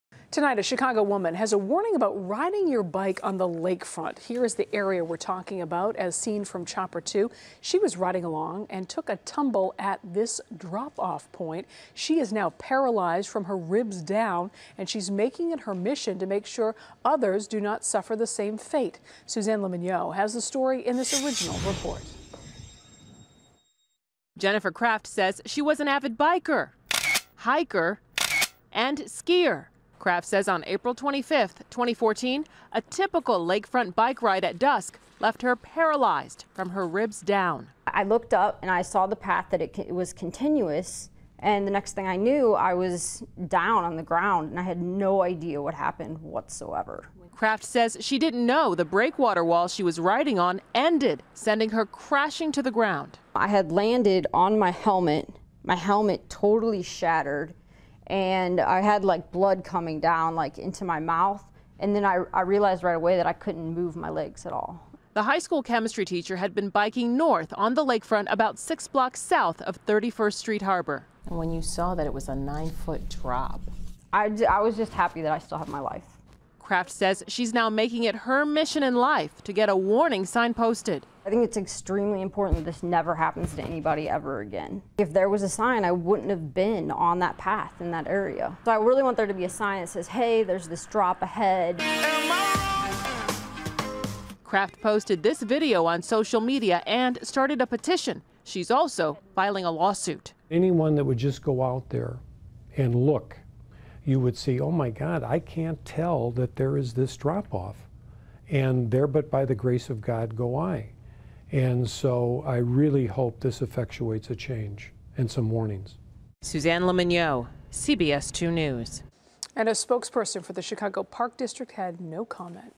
Paralyzed Bicyclist Sues Over Dangerous Dropoff Along Lake
Jennifer Kraft says a warning sign would have saved her from a life-altering injury on Chicago's lakefront. CBS 2's Suzanne Le Mignot reports.